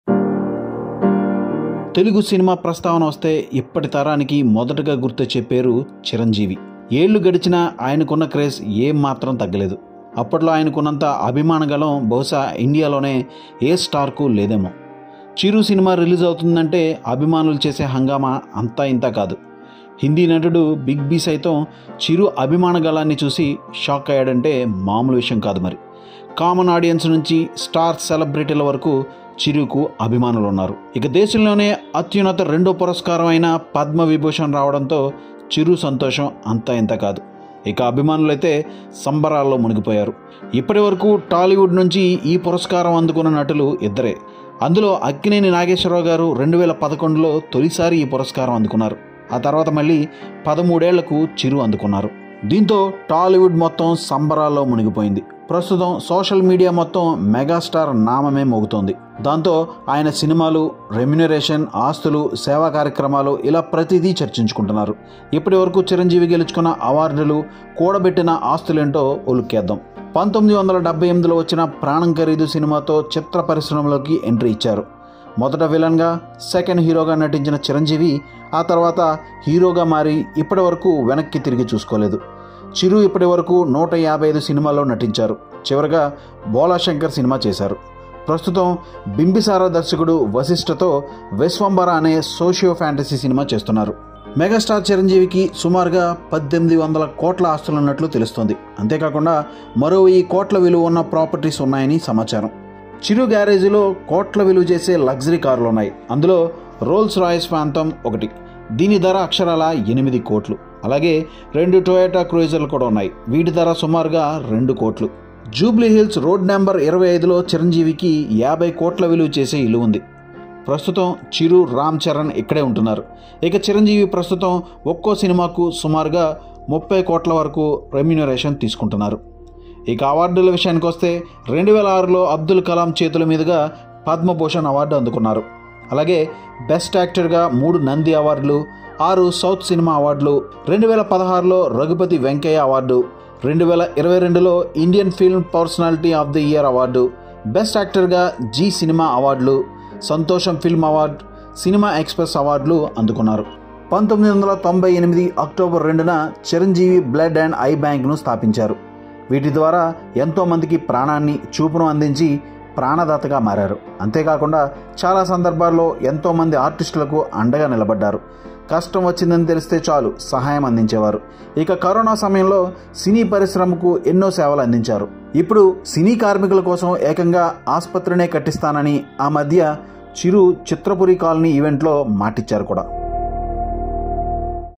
మెగాస్టార్ చిరంజీవి ఆస్తుల విలువ | Megastar Chiranjeevi Assets, Luxury House, Cars, Income, Services
మెగాస్టార్ చిరంజీవి ఆస్తుల విలువ | Megastar Chiranjeevi Assets, Luxury House, Cars, Income, Services, Net worth Full Details